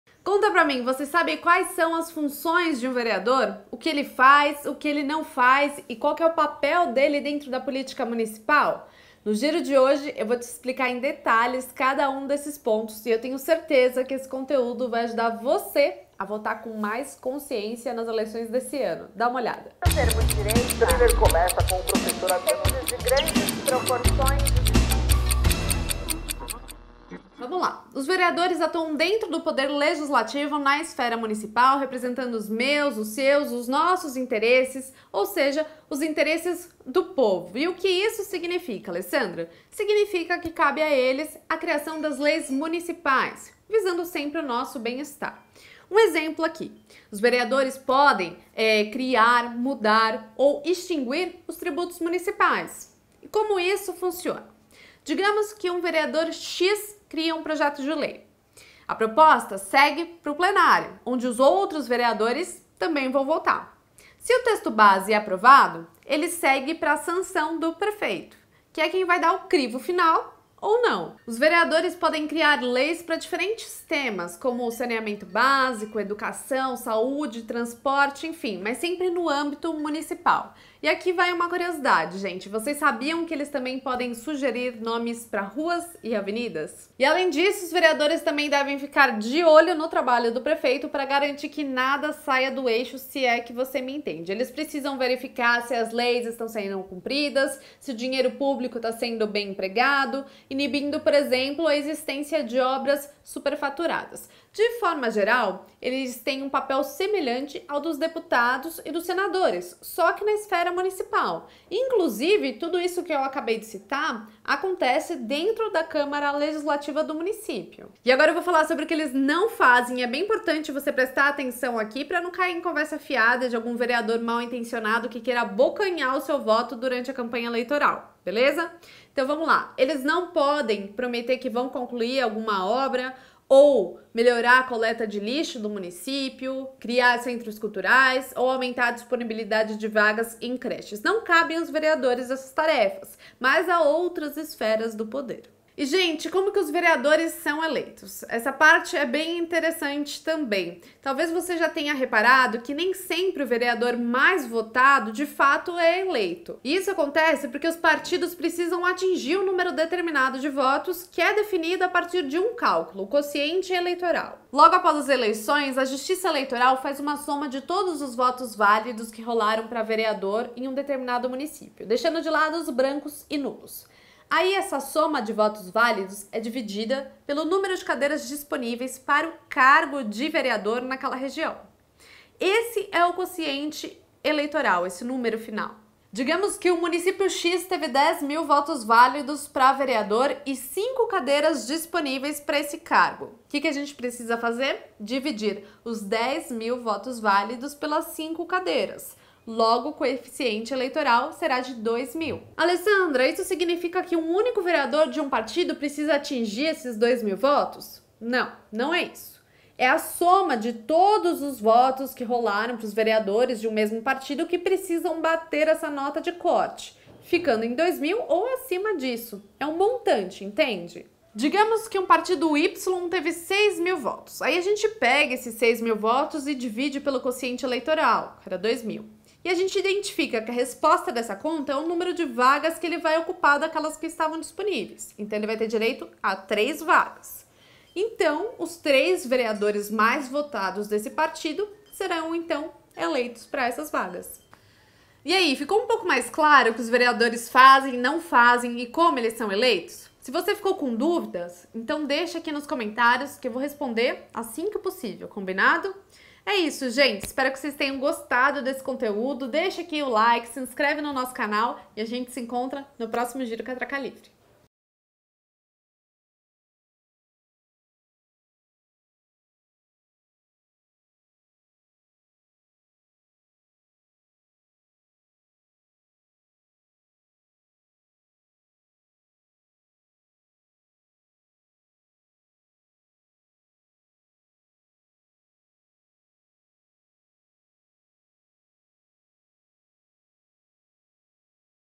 Eleições 2020: o que faz um vereador?
Você sabe quais são as funções de um vereador? O que ele faz? Não faz? A importância dele dentro da política municipal?

Os vereadores são membros da Câmara Municipal, eleitos por voto popular para um mandato de quatro anos.

Eles atuam como os representantes do povo no Poder Legislativo, criando, alterando ou extinguindo leis que devem priorizar o bem-estar e os interesses da população.

Além disso, os vereadores também precisam acompanhar o trabalho do prefeito para verificar se as leis estão sendo cumpridas e o dinheiro público bem empregado, inibindo, por exemplo, a existência de obras superfaturadas.

De forma geral, eles exercem um papel semelhantes ao dos deputados e senadores, só que no âmbito municipal.

Neste ano, os eleitores dos 5.570 municípios do Brasil deverão escolher seus representantes para o poder público municipal, ou seja, prefeitos, vice-prefeitos e vereadores. Por isso, entender quais as funções e responsabilidade de cada um desses agentes públicos é fundamental!

Solta o play e confira no vídeo mais detalhes sobre esse assunto!

Veja +:

Eleições 2020: ausência deve ser justificada pelo aplicativo e-Título

Eleições 2020: TSE aponta recorde de candidatos negros e mulheres